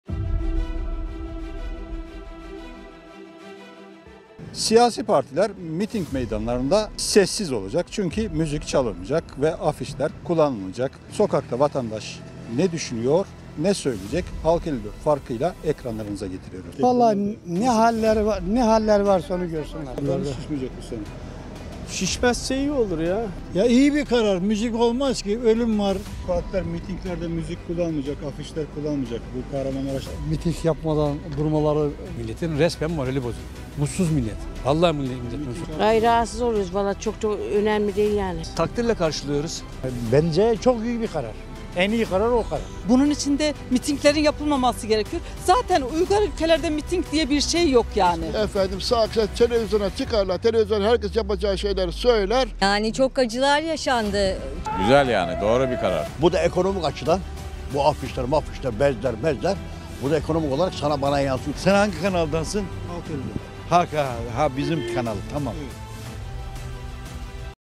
Sakarya seçim meydanlarında müzik kullanılmayacak sesiz mitinkiler yapılacak
Sakarya14 mayıs 2023 milletvekili seçimi ve cumhurbaşkanı seçiminde meydanlarda müzik kullanılmayacak sesiz mitinkiler yapılacak.